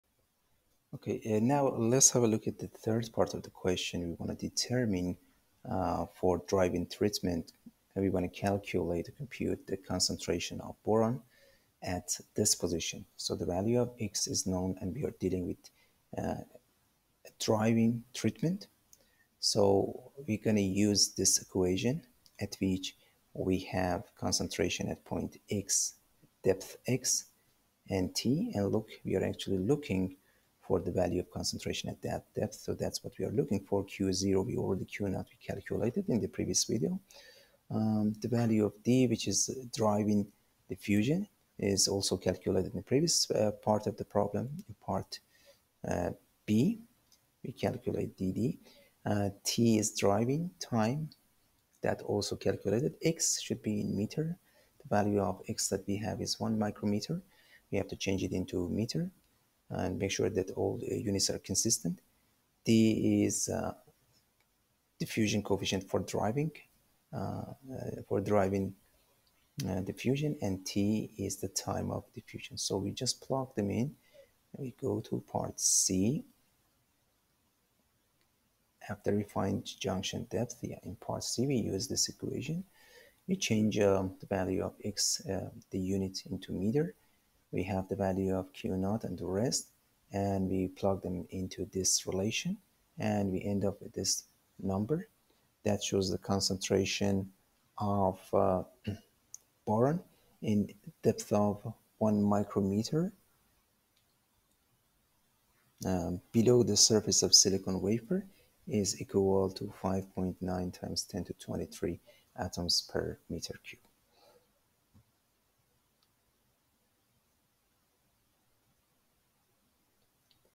Example of diffusion in semi-conducting materials - Part 2
Example 7.6 Callister
Boron atoms are to be diffused into a silicon wafer using both predeposition and drive-in heat treatments; the background concentration of B in this silicon material is known to be 1x10^20 atoms/m3. The predeposition treatment is to conducted at 900oC for 30 minutes; the surface concentration of B is to be maintained at a constant level of 3x10^26 atoms/m3. Drive-in diffusion will be carried out at 1100oC for a period of 2 h. For the diffusion coefficient of B in Si, values of Qd and Do are 3.87 eV/atom and 2.4x10^-3 m2/s, respectively.

Calculate the value of Q0
Determine the junction depth